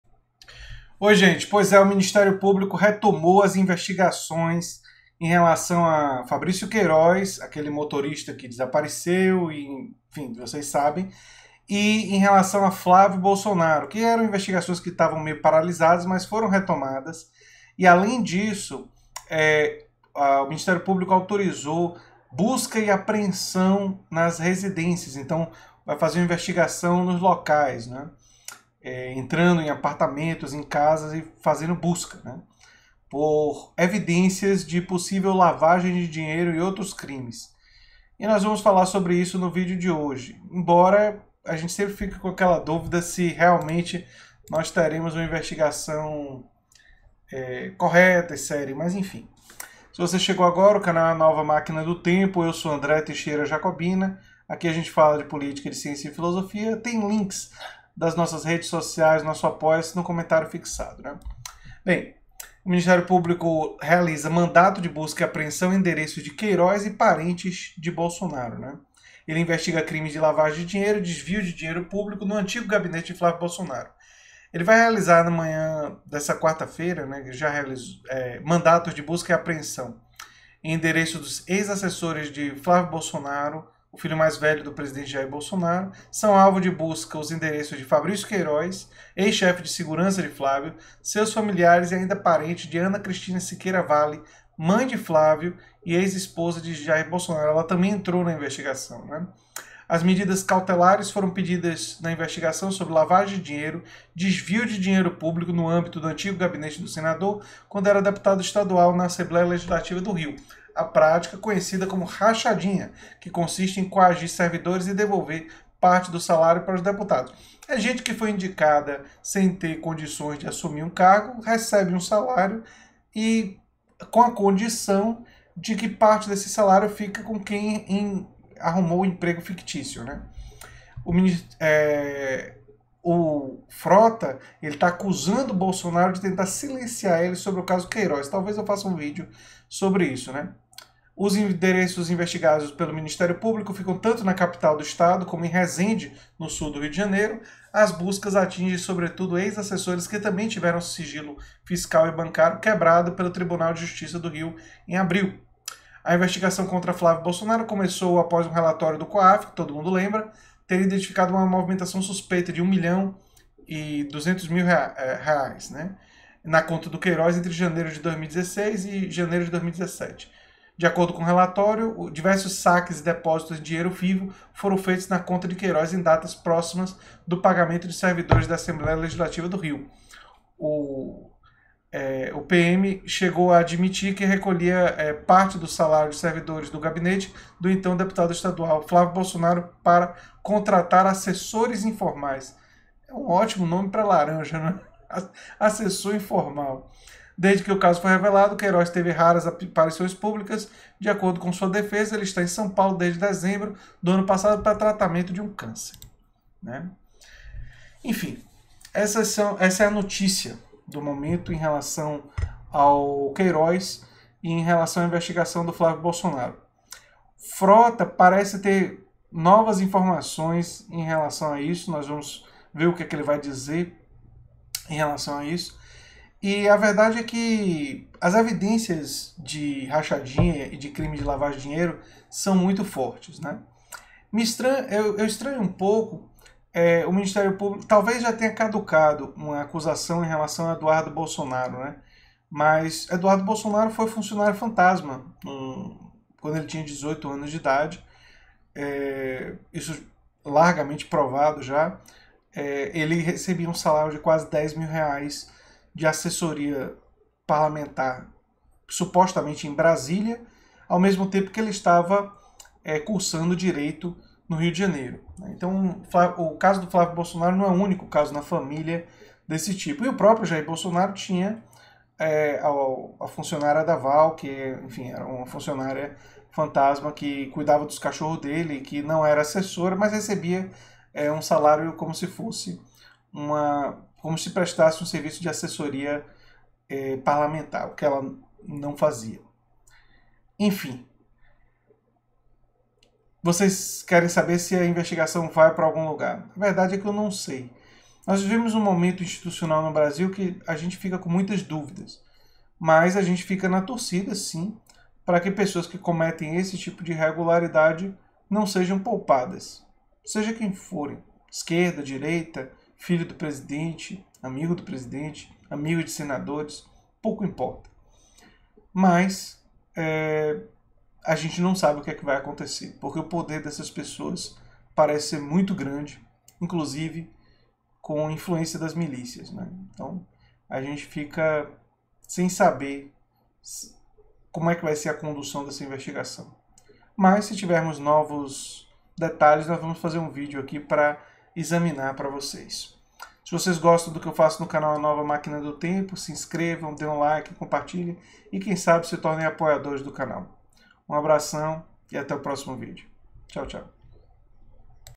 BUSCA E APREENSÃO EM ENDEREÇOS DE QUEIROZ E PARENTES DE BOLSONARO
Análise e opinião sobre essa questão.